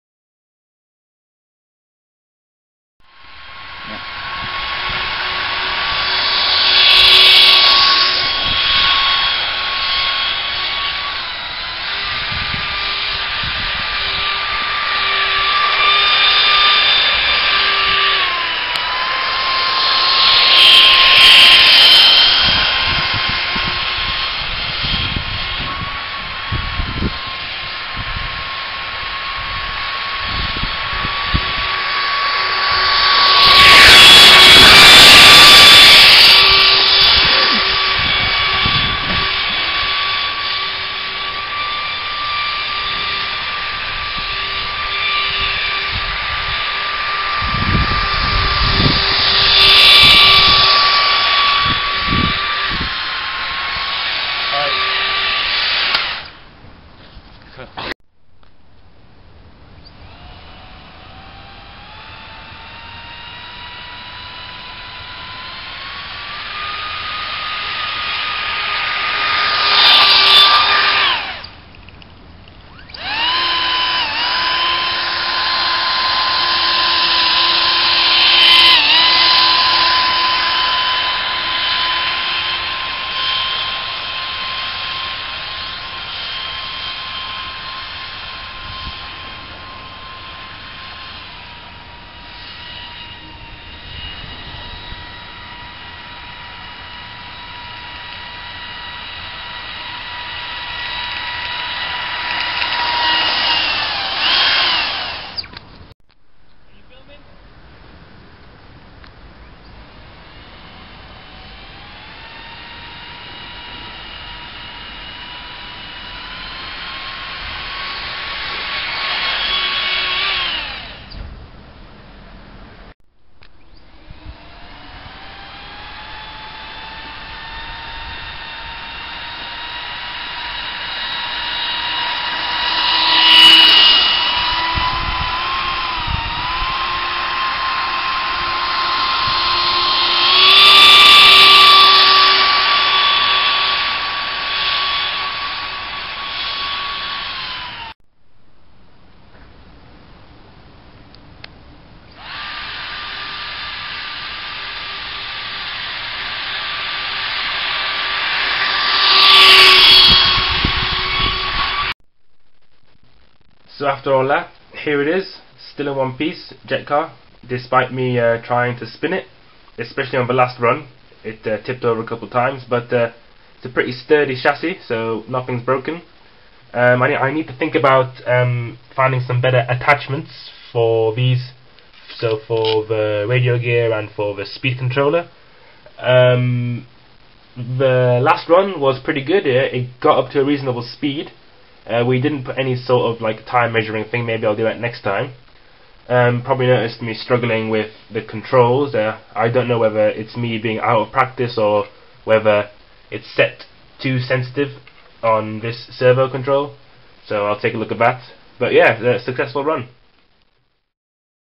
Ducted Fan JetCar outdoor testing, spins and crashes
Having found some free time I got the chance to finally take JetCar out for an outdoor testing session. I found a reasonably long and quiet stretch of road and drove it around for a while. I was very pleased with it in general, good speed, strong frame. It was a damp road so there was little grip and it tended to spin (spinning was also caused by me), but all in all a good set of runs.

Parts recap:
-Mardave chassis
-500W Brushless Motor
-ELF hobby 30A ESC
-Walkera 0701 and 701 radio gear